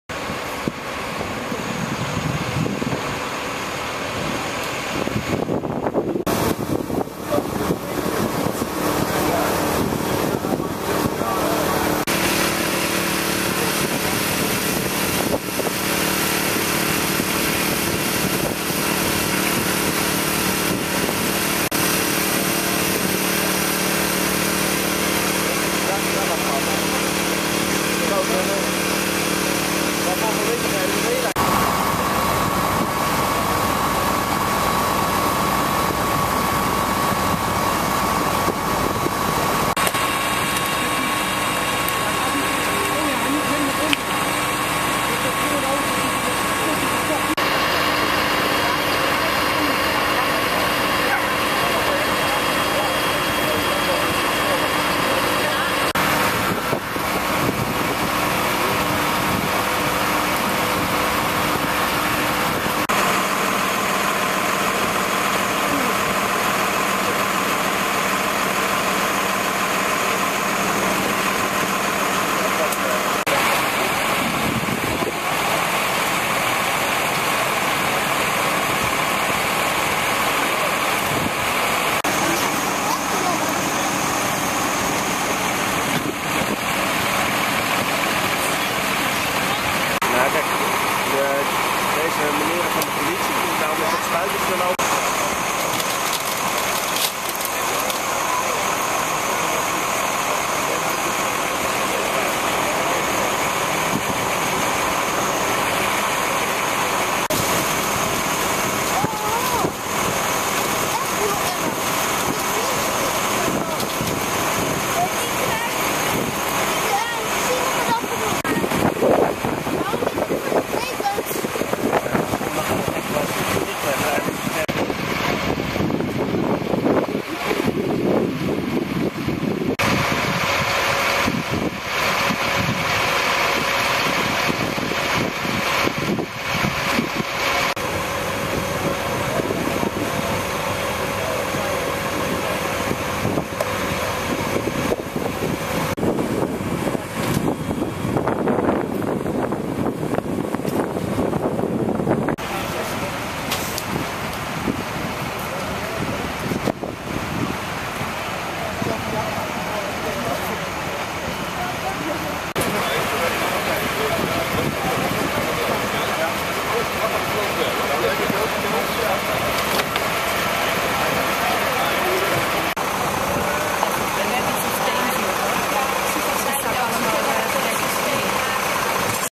Zwaar ongeval bij De Campen in Houten
Op de Rondweg ter hoogte van de kruising met de Campen is zaterdagavond 7 januari even voor 19.00 uur een flinke aanrijding gebeurd. Twee mensen raakten bekneld en moesten door de brandweer worden bevrijd. De aanrijding gebeurde op een kruising waar verkeerslichten staan.

Uit de eerste berichten ter plaatse blijkt een auto op de Rondweg, komende uit de richting van De Slagen, door het rode licht te zijn gereden. Daarbij werd een auto komende uit de wijk De Campen vol in de zijkant geraakt. Aan de schade te zien is de aanrijding behoorlijk geweest. Een auto is zeker total loss en de andere waarschijnlijk ook.